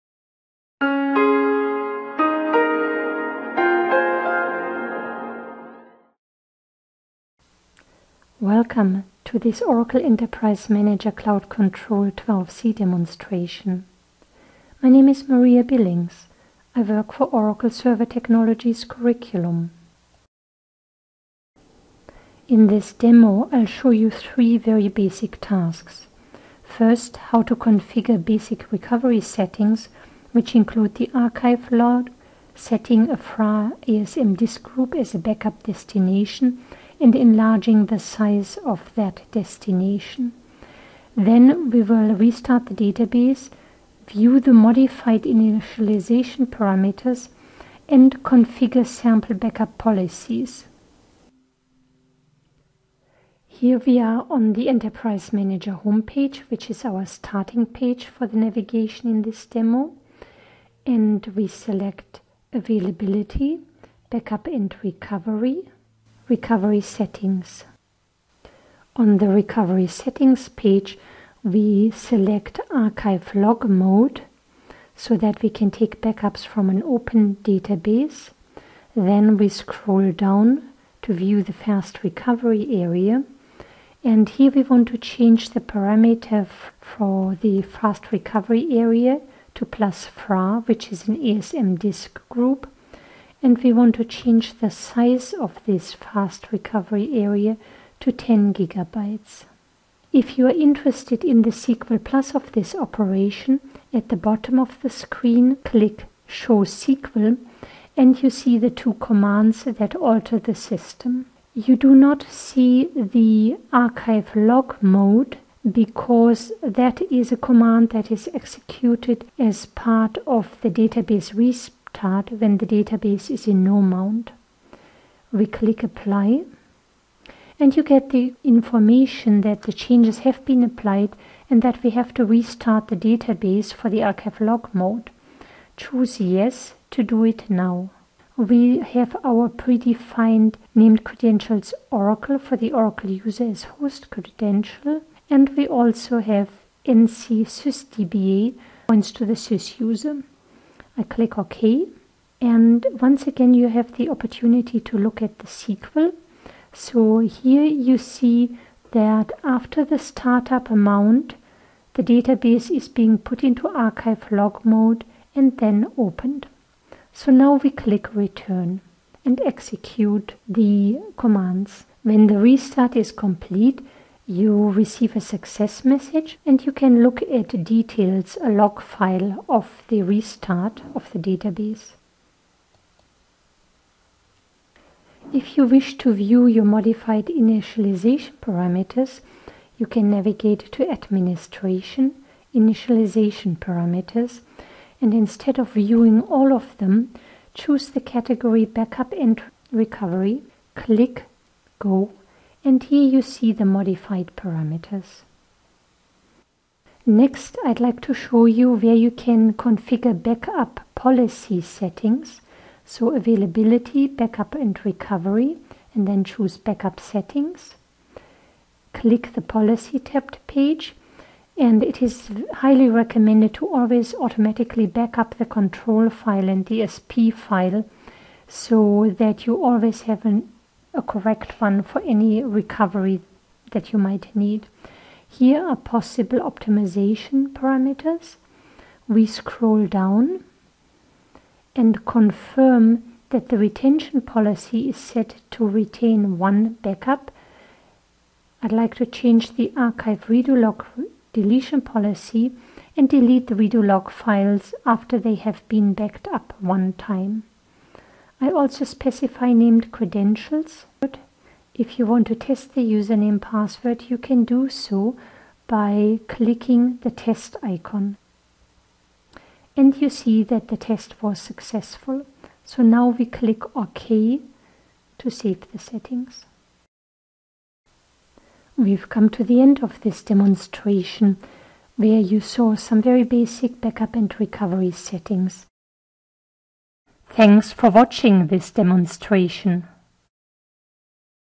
Configuring Basic Backup and Recovery Settings for a Database in Cloud Control 12c
This video shows you how to configure basic backup and recovery settings for an Oracle Database 12c using Oracle Enterprise Manager Cloud Control 12c.